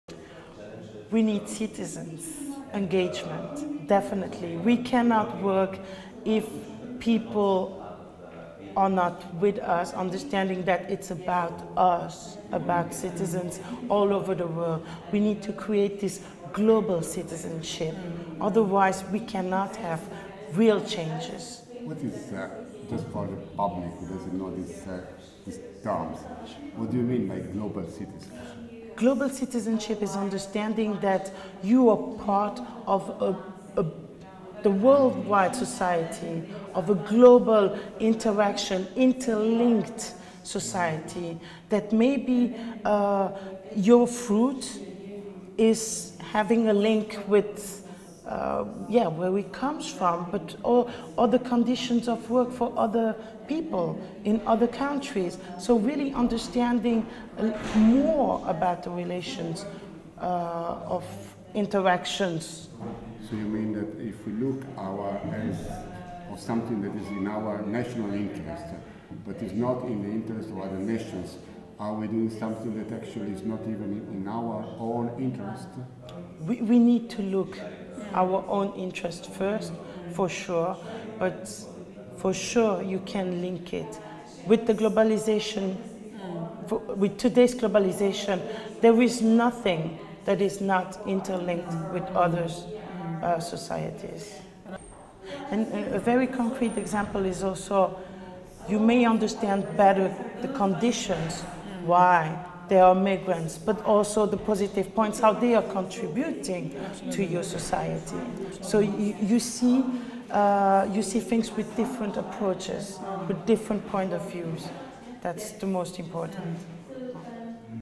Etikwa Ikutu - We need the global citizenship approach
Etikwa Ikutu
Member of the European NGO confederation for relief and development.